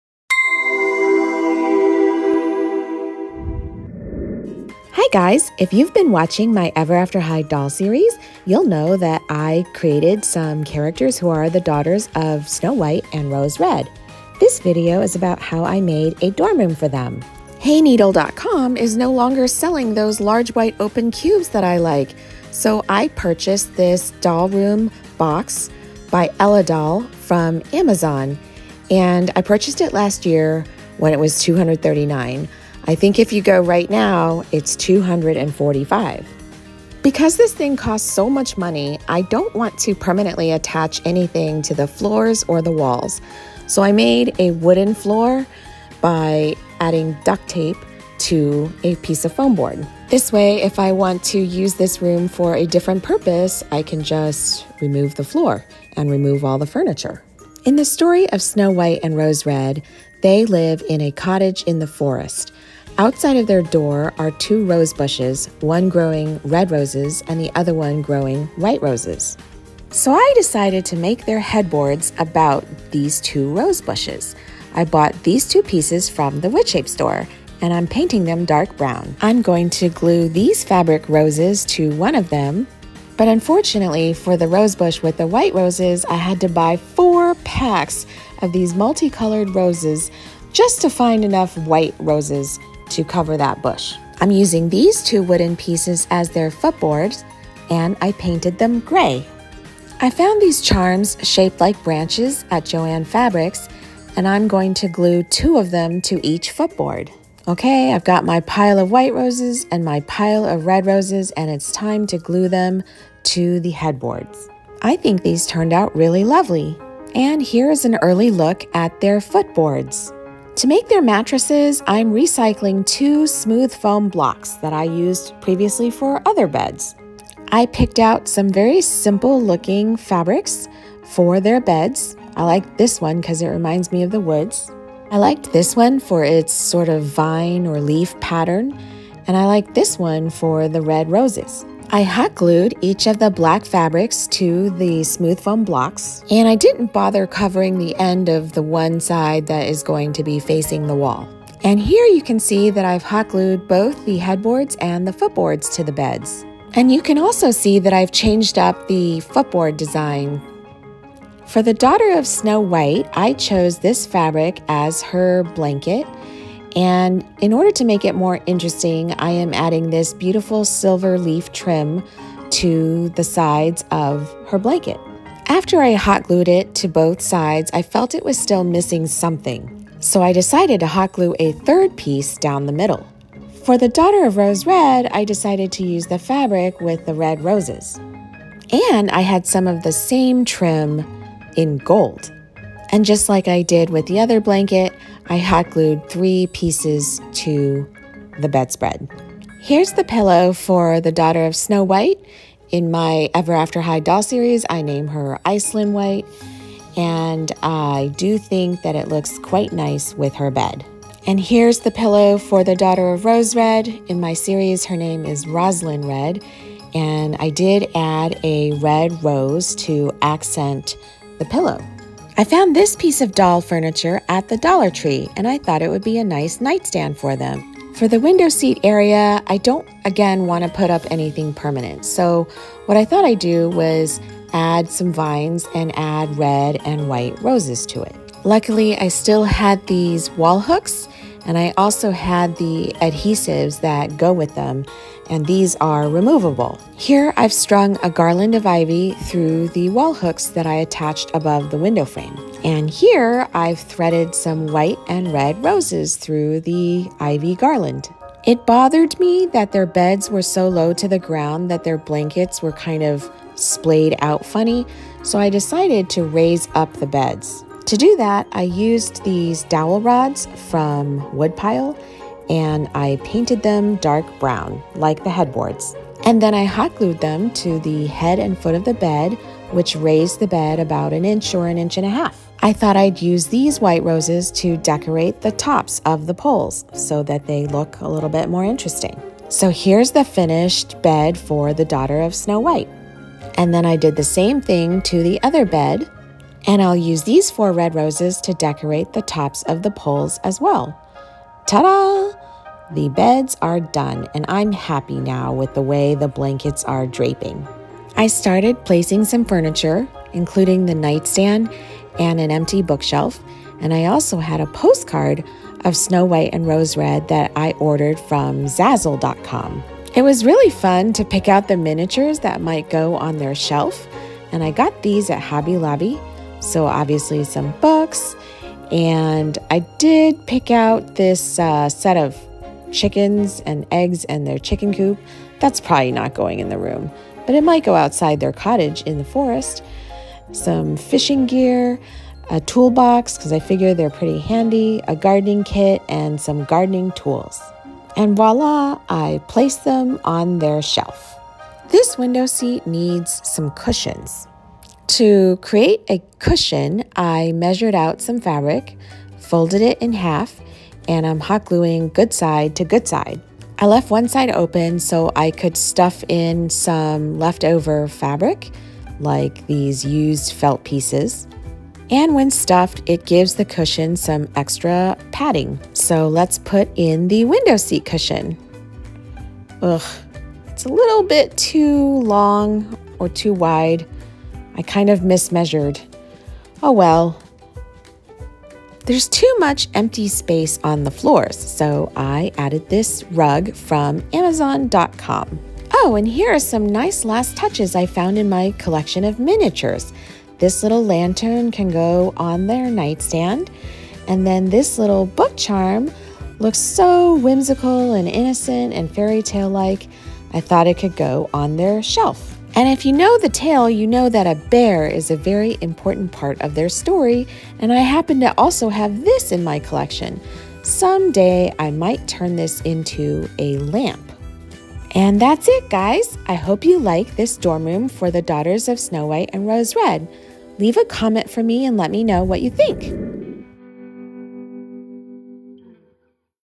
AN EVER AFTER HIGH DOLL DORM ROOM FOR THE DAUGHTERS OF SNOW WHITE AND ROSE RED
After last week's episode of my Ever After High doll series, "More Than Just a Fairy Tale," a subscriber asked if I could make a video about how I had created the room. This is the video that shows that - beware, this dorm room is not like the others! The doll room box was so expensive that I decided to make the room "reusable." Meaning nothing permanent got attached to the floors or the walls, and I can pull it down at any time to accommodate a new idea.

I also created my own custom Ever After High characters, Iclyn White and Roslin Red, the daughters of Snow White and Rose Red, also by the Grimm brothers.